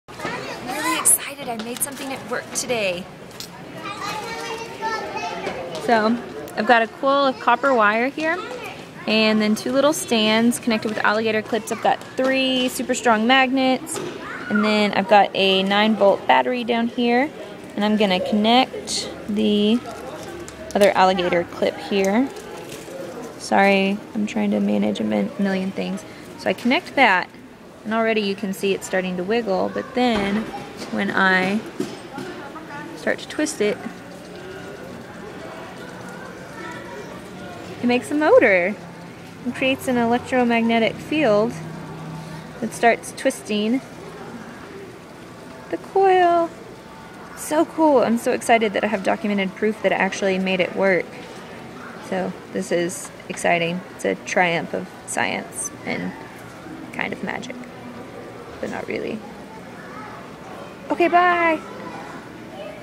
Electromagnet Motor Experiment
Science!

This is my electromagnet motor experiment I did at work, Saturday, November 12, 2011.